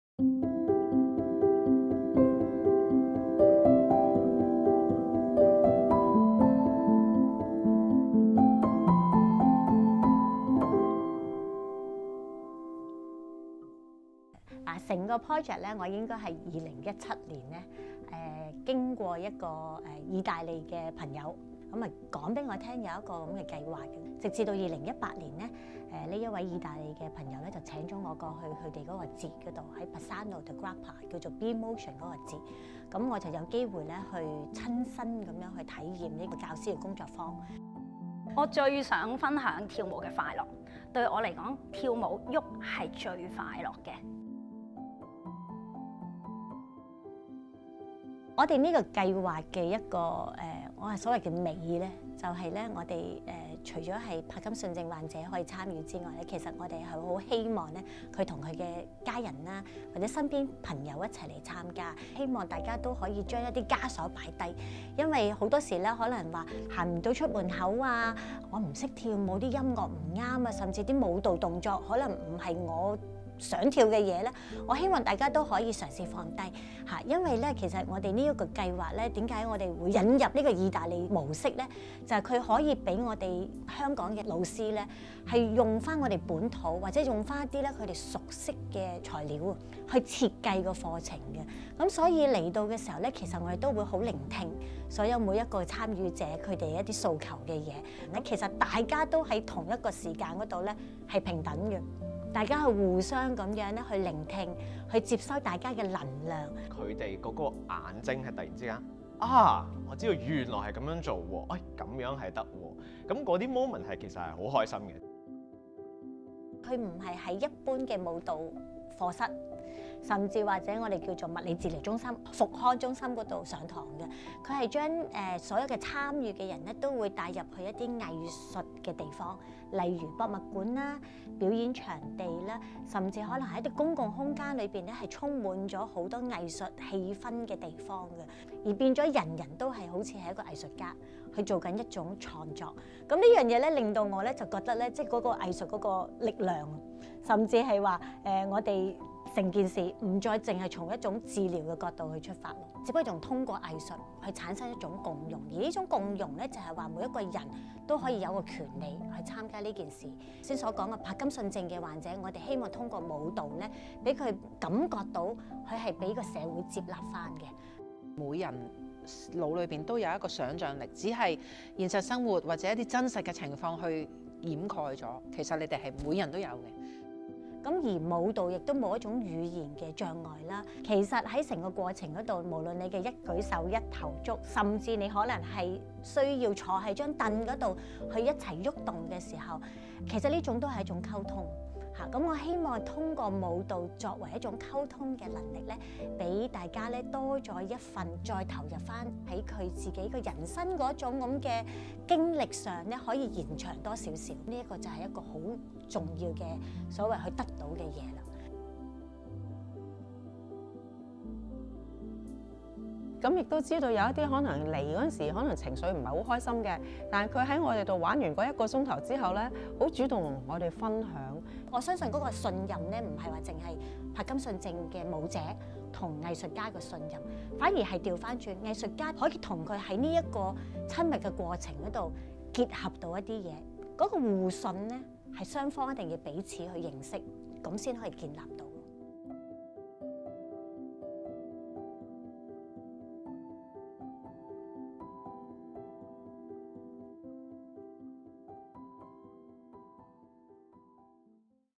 Introductory video of Jockey Club Dance Well Project | 賽馬會「觸動」舞蹈計劃短片
The Jockey Club Dance Well Project is a creative movement programme designed for people with Parkinson’s disease and people of different ages and abilities through regular dance classes and activities in artistic spaces like galleries, museums and performance venues. Organised by the School of Dance of The Hong Kong Academy for Performing Arts (HKAPA) and funded by The Hong Kong Jockey Club Charities Trust, this three-year Project will launch the Dance Well classes in September 2022.

Learn more about the Project:  jcdancewell.hkapa.edu

賽馬會「觸動」舞蹈計劃是一個為柏金遜症患者以及其他不同年齡和身分的參加者而設的創意舞蹈項目，於畫廊、博物館和表演場地等藝術空間提供定期的創意舞蹈體驗。項目為期三年，由香港演藝學院（演藝學院）的舞蹈學院主辦、香港賽馬會慈善信託基金捐助，將於2022年9月啟動，推出「觸動」舞蹈課。

了解計劃詳情： jcdancewell.hkapa.edu